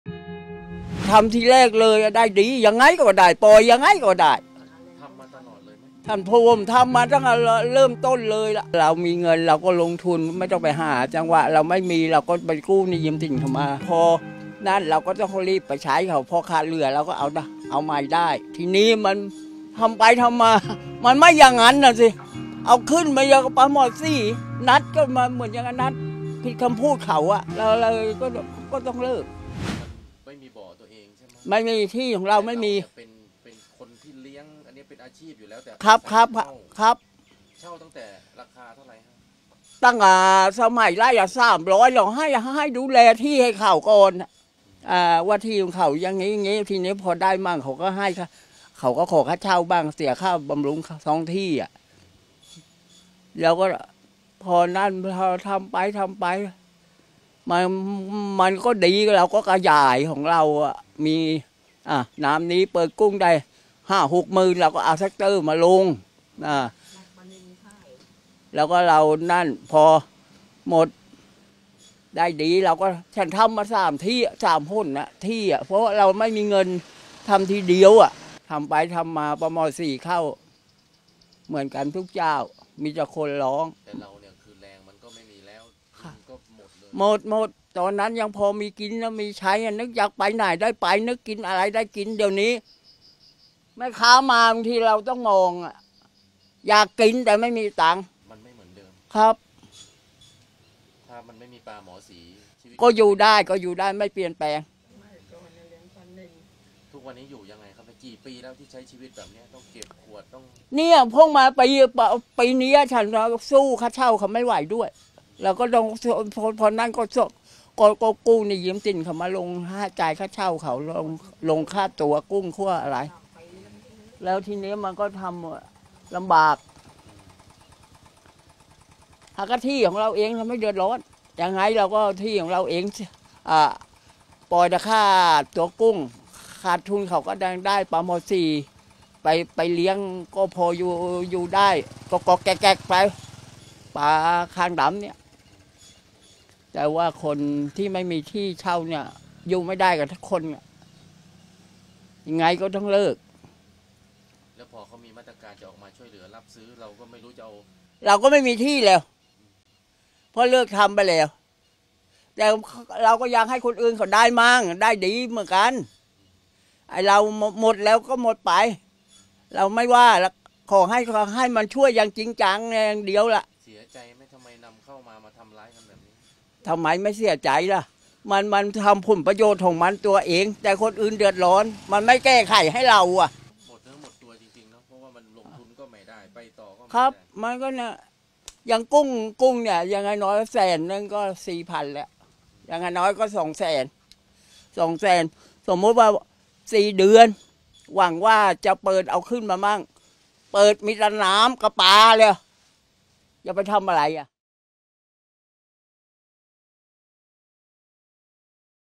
ปลาหมอคางดำพลิกชีวิต จากทำบ่อเลี้ยงกุ้ง 20 ไร่ สู่เก็บขวดขายประทังชีวิต | Local News
สุดช้ำ “ลุงรื่น” อดีตผู้เลี้ยงกุ้งใน จ.สมุทรสงคราม เปิดใจ ปลาหมอคางดำทำชีวิตเปลี่ยน จากทำบ่อเลี้ยงกุ้ง 20 ไร่ สู่เก็บขวดขายประทังชีวิต

📰 ติดตามข่าวสารเพิ่มเติมได้ที่